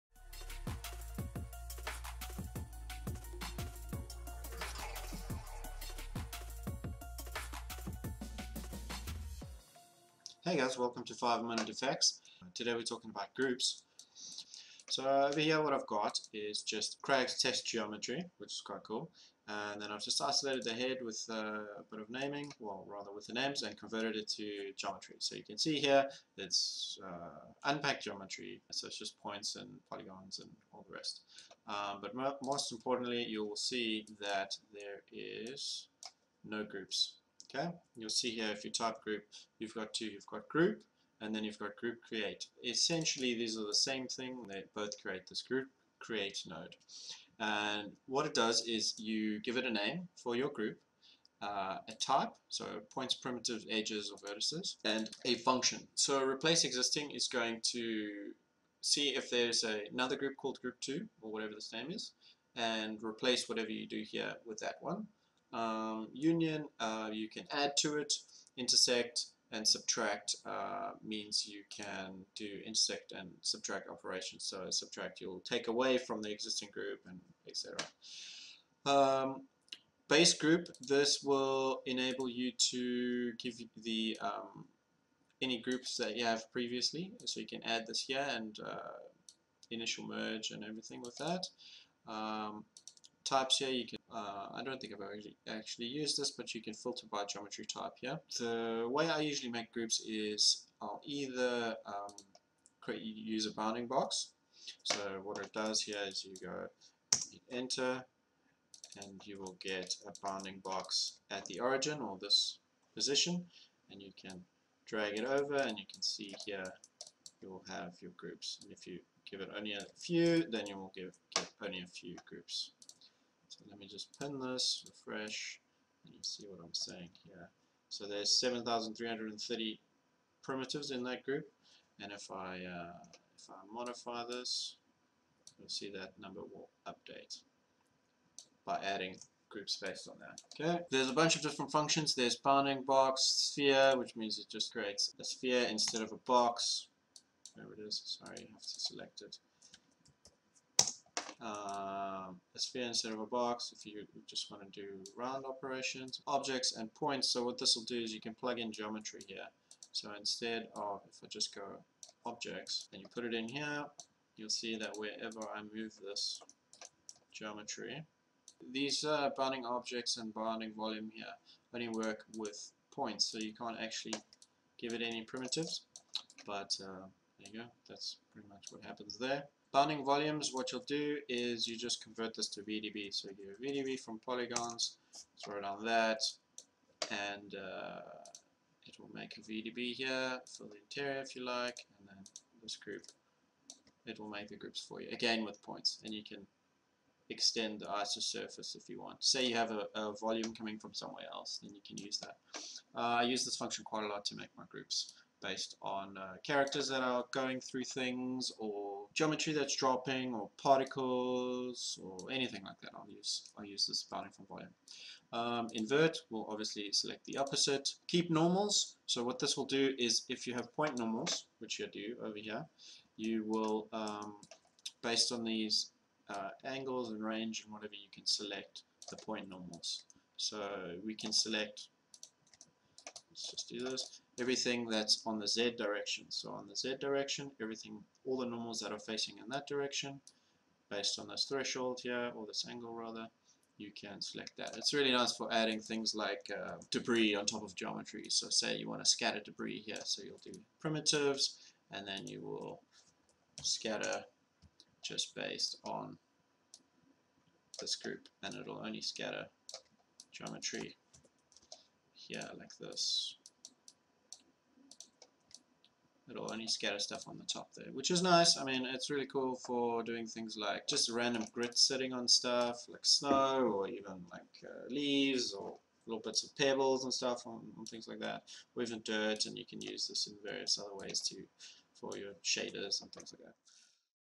Houdini Groups In 5 Minutes - Part 1
Houdini Groups In 5 Minutes - Part 1 is a quick 5 minute video showing you all about groups and advanced grouping techniques in Sidefx Houdini 17. In this video I run through the different group nodes available and the various settings that I mostly use. This video is aimed and beginner to mid level users.